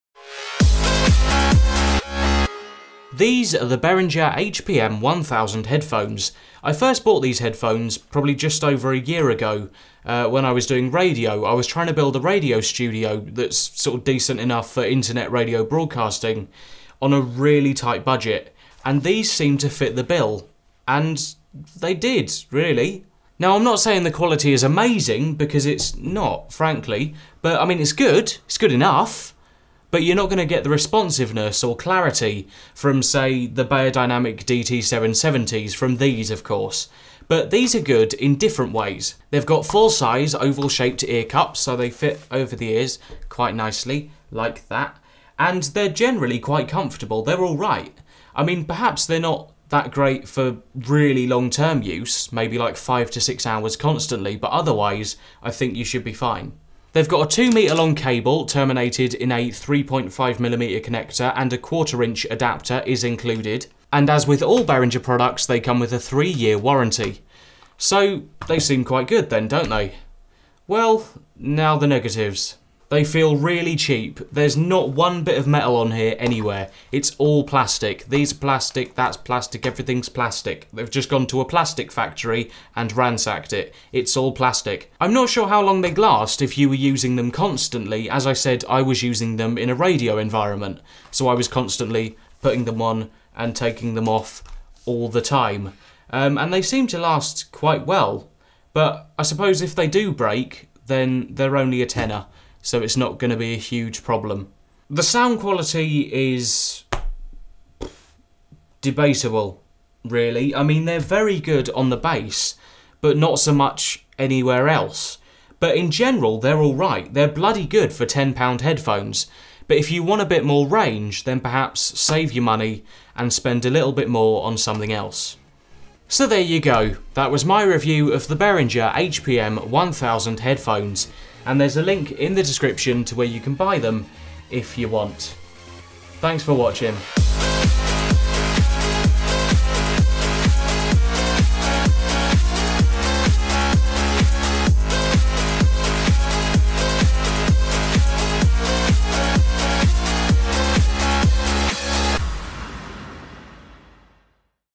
Behringer HPM1000 Headphones Review
They cost about £10. But are they any good? Let's find out!